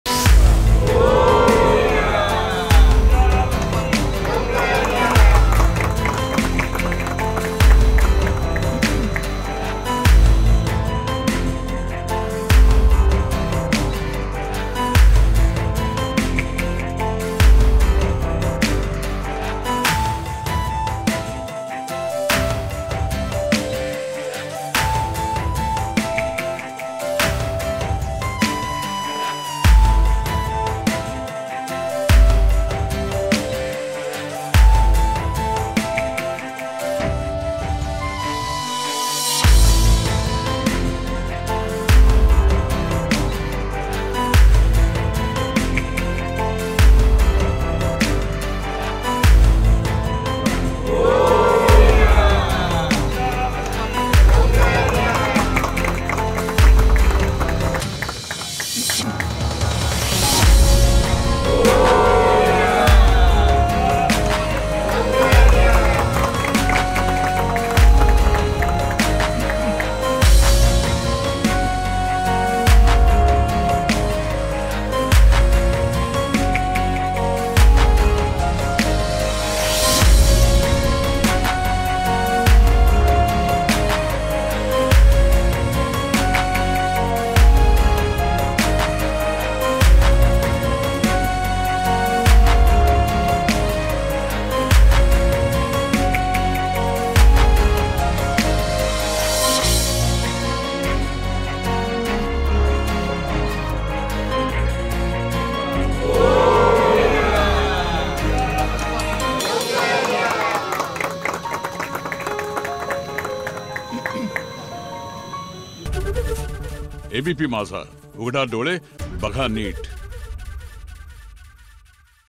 Kolhapur Ganpati Visarjan उत्सवाला तंत्रज्ञानाची जोड, अत्याधुनिक पद्धतीनं होतंय बाप्पाचं विसर्जन
ABP Majha LIVE TV | Maharashtra Cabinet | Cyrus Mistry Accidental Death |  Marathi News 24/7

Video Credit : Producer - Sudhir Kakde | Editor - Saurabh